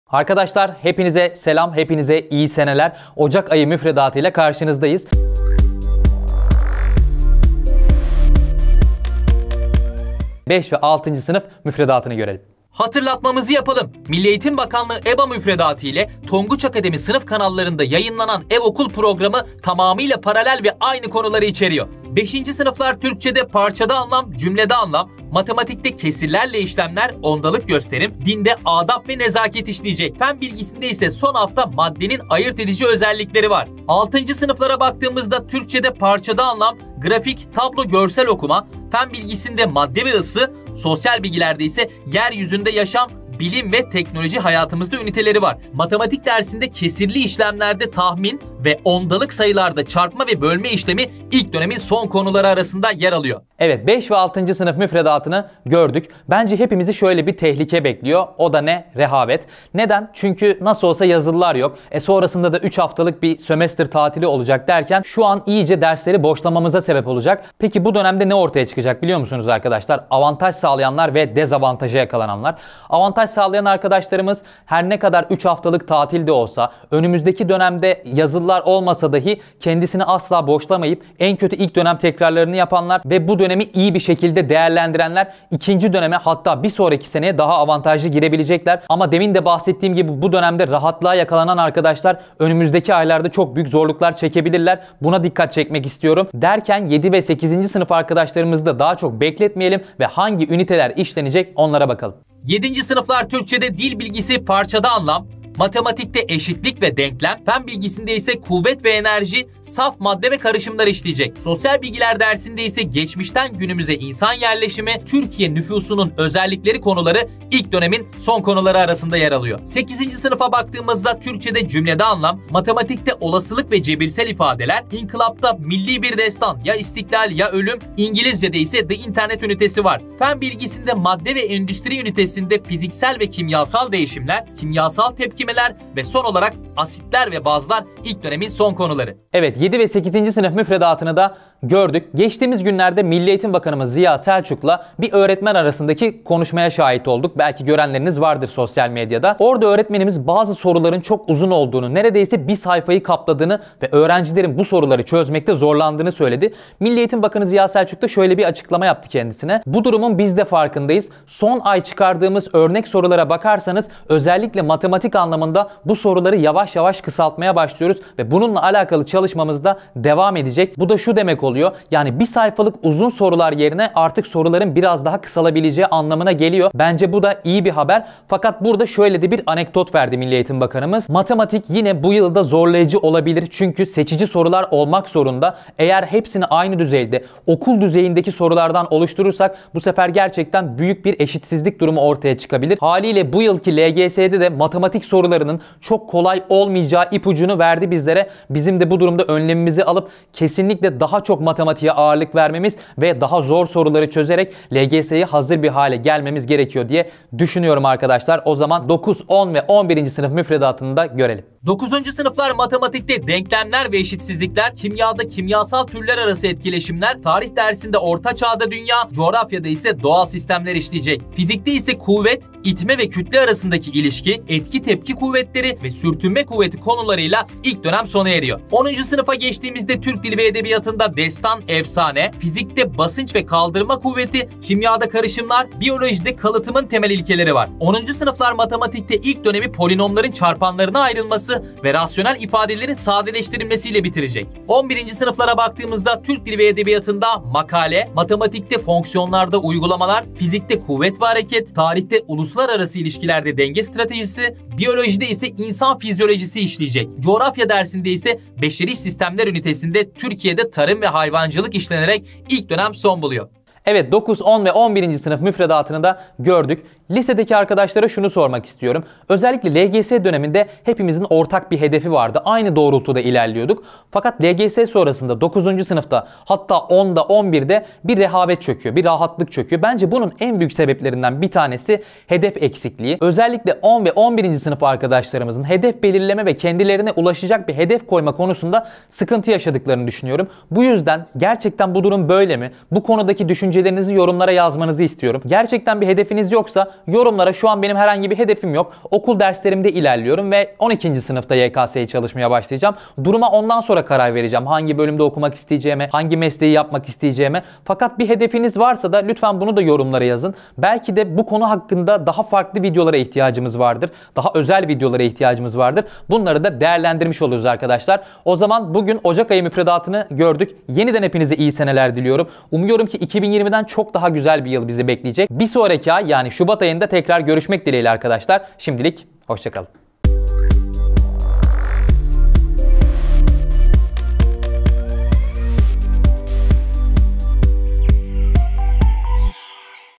Ocak Ayı Müfredatında Hangi Konular Var? 📚🗓
Uzaktan eğitim sürecinde Ocak ayı boyunca hangi konulardan sorumlusunuz? Dikkat etmeniz gereken konular neler? 5'ten 11. sınıfa kadar sizler için anlattık!🤓
Giriş- 00:00
5. Sınıf Müfredatı: 00:22
6. Sınıf Müfredatı : 00:33
7. Sınıf Müfredatı : 01:42
8. Sınıf Müfredatı : 02:00
9. Sınıf Müfredatı : 03:33
10. Sınıf Müfredatı : 03:51
11. Sınıf  Müfredatı: 04:05